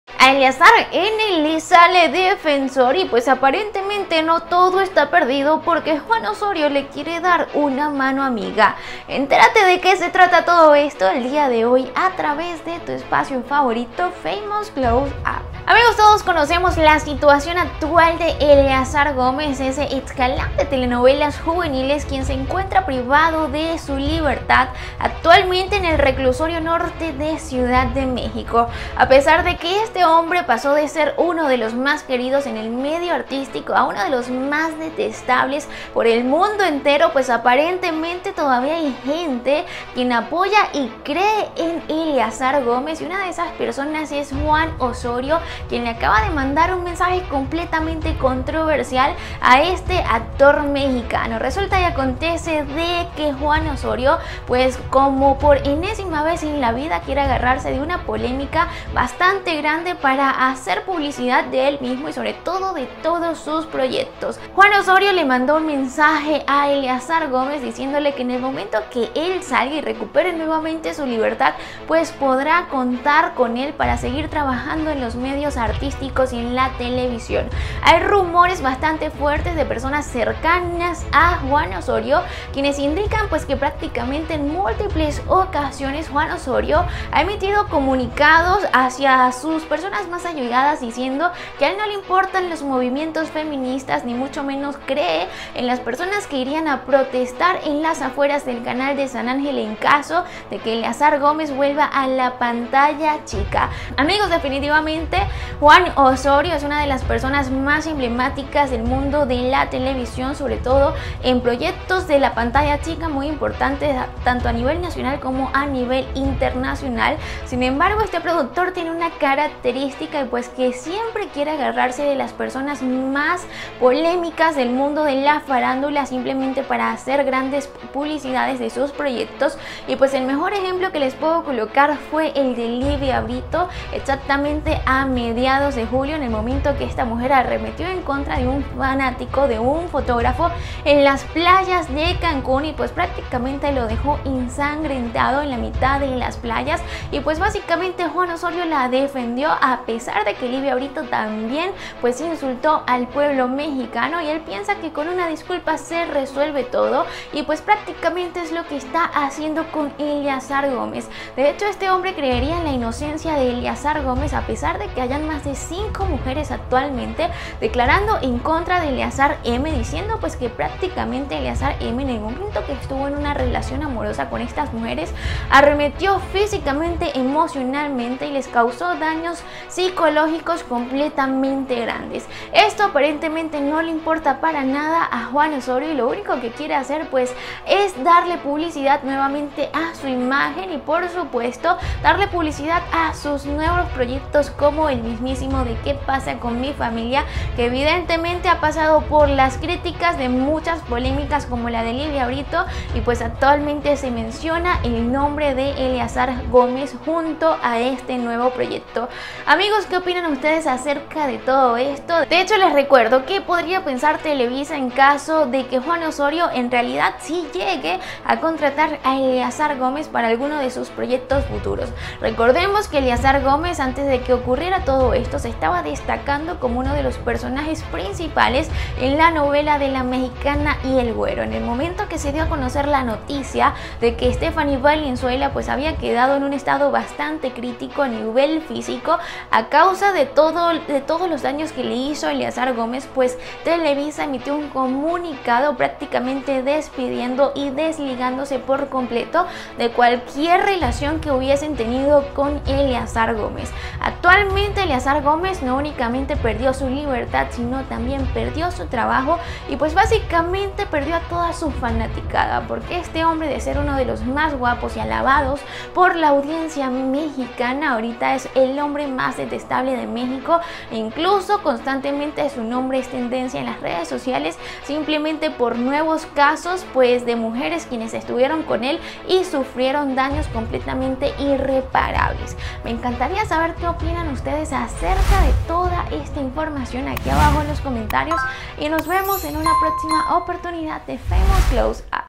🚨💥 Juan Osorio 𝗹𝗲 𝗱𝗮 𝗨𝗡 𝗥𝗘𝗦𝗣𝗜𝗥𝗢 a Eleazar N 🤝🔥
El productor Juan Osorio dio su opinión en torno al caso de Eleazar 'N', actor con el que trabajó en algunas producciones